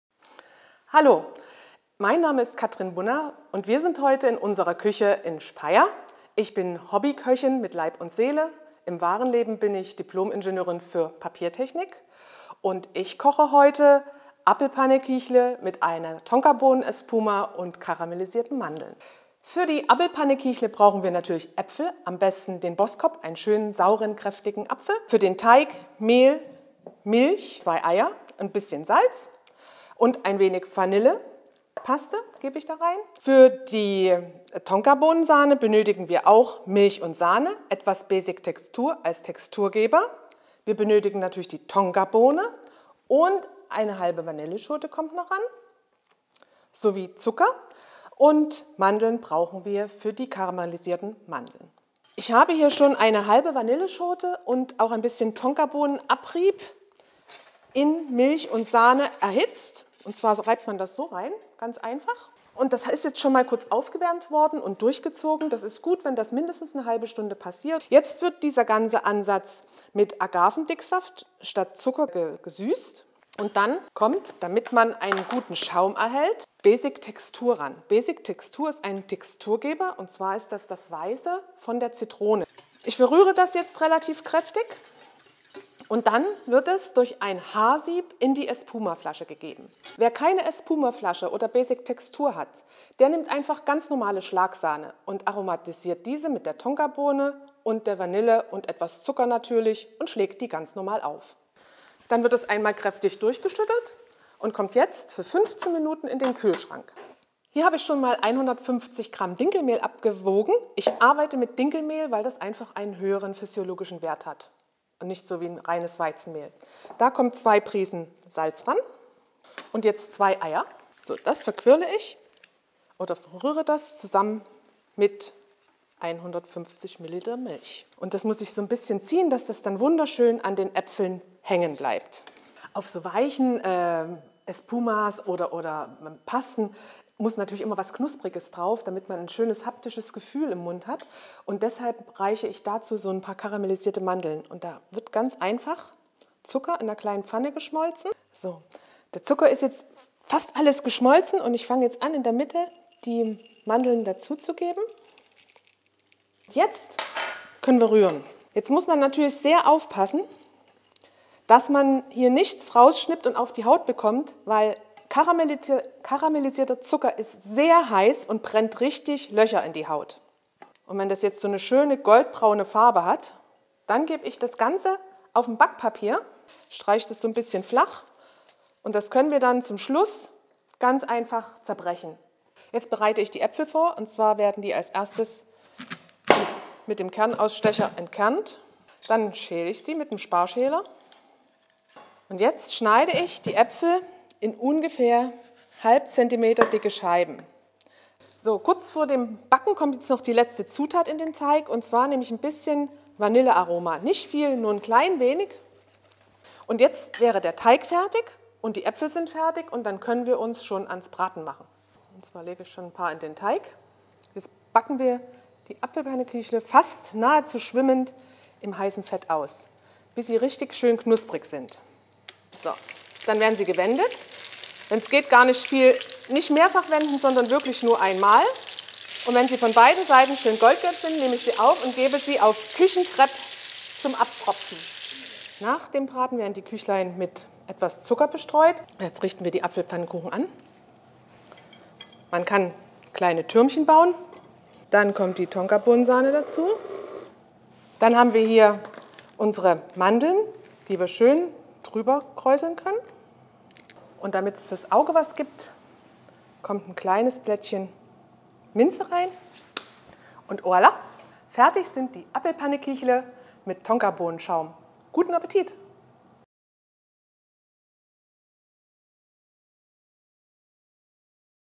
Katrin Bunners Rezept für Apfelküchlein
Foodbloggerin Katrin Bunner aus Speyer ist Hobbyköchin mit Leib und Seele, wie sie sagt. Auf ihrem Blog "Katrin kocht" teilt sie mit anderen Hobbyköchen die unterschiedlichsten Rezepte. Für DIE RHEINPFALZ hat sie etwas Süßes aufgetischt: "Abblpannekiechle" wie Apfelküchlein in der Pfalz heißen. Dazu gibt es Vanille-Tonkabohnenschaum und karamellisierte Mandeln.